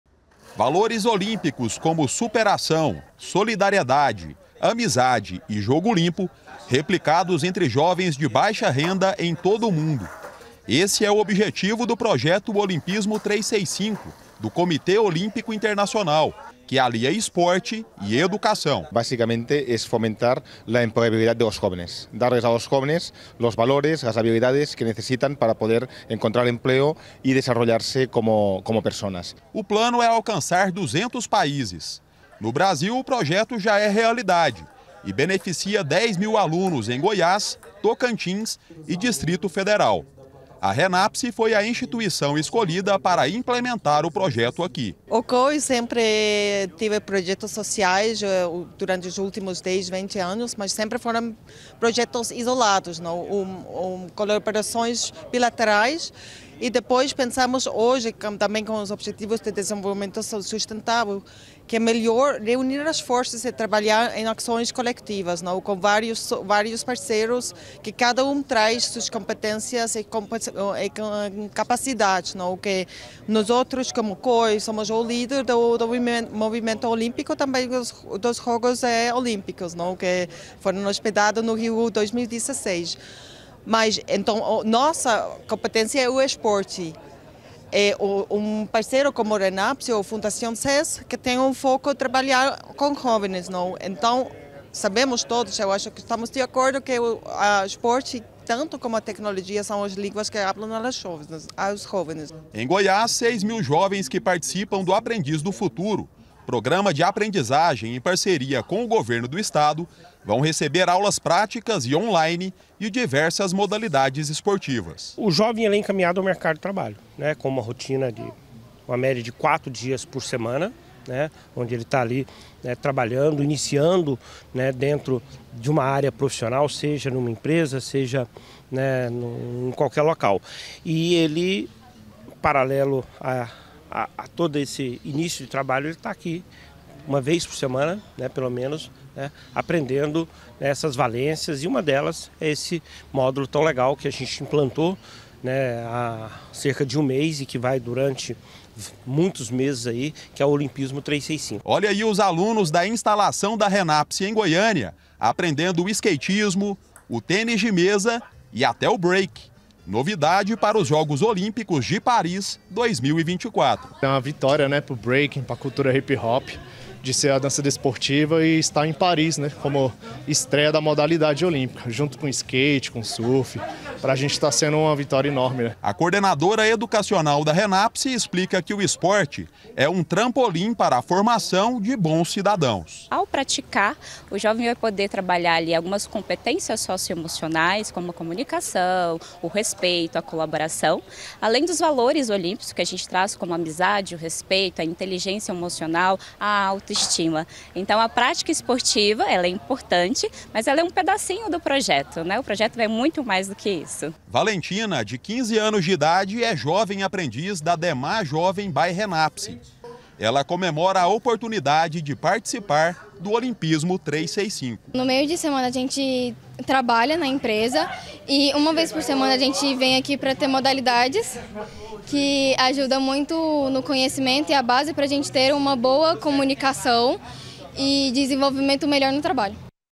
ESPORTE CIDADÃO: INICIATIVA DO COI BENEFICIA JOVENS CARENTES
UMA INICIATIVA DO COMITÊ OLÍMPICO INTERNACIONAL QUE VISA A FORMAÇÃO HUMANA DOS JOVENS ATRAVÉS DA PRÁTICA ESPORTIVA ESTÁ CHEGANDO AO BRASIL O PROJETO JÁ É DESENVOLVIDO EM VÁRIOS PAÍSES E DEVE ATINGIR 10 MIL JOVENS CARENTES AQUI NO BRASIL.